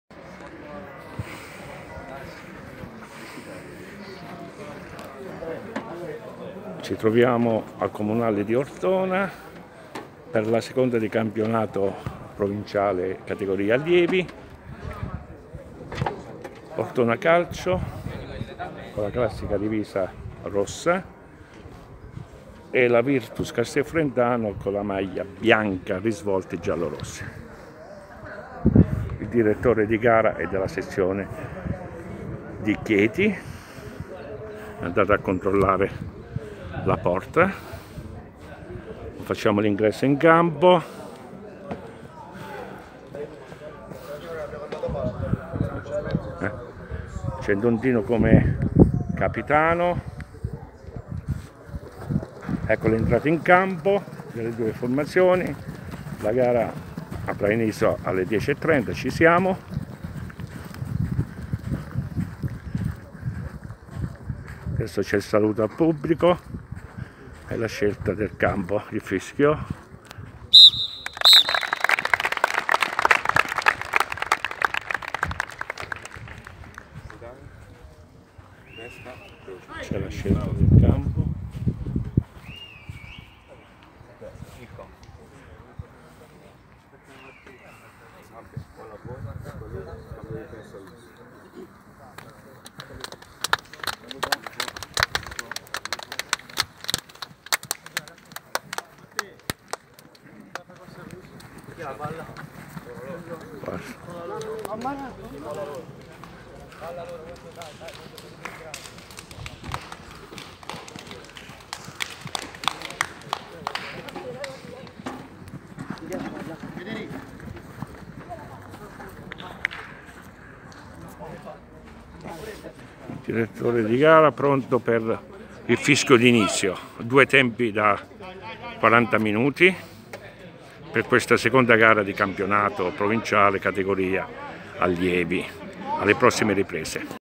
VIRTUS CASTEL FRENTANO " ALLIEVI " Stag. 2018-2019
Domenica  scorsa nella seconda gara di Campionato ,i nostri ragazzi degli ALLIEVI d Mister Giovanni DI GIULIO  sono stati sconfitti in trasferta a Ortona  dai coetanei dell'Ortona Calcio per 4 - 1.
Nostro Marcatore Riccardo DEL BELLO .